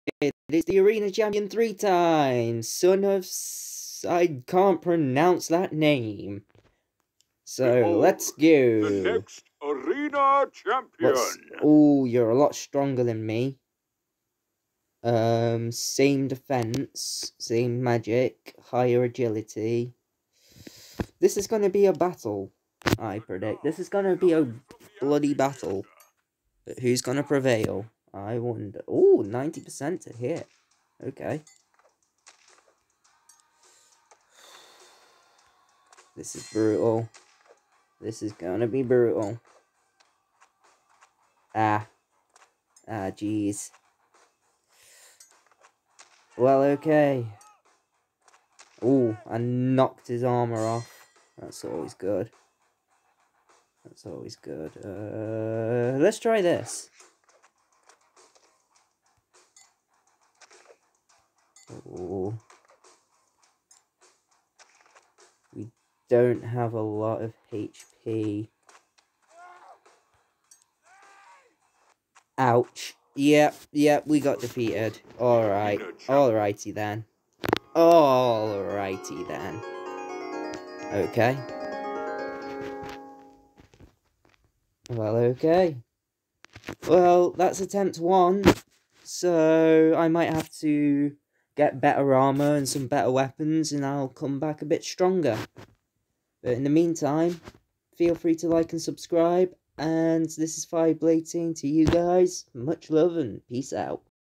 Fireblades Vs the Son of Stylonius
Its time to face the arena champion 3: The  Son Son of Stylonius! Can I: Fireblades the Hitman take out the Son of Stylonius?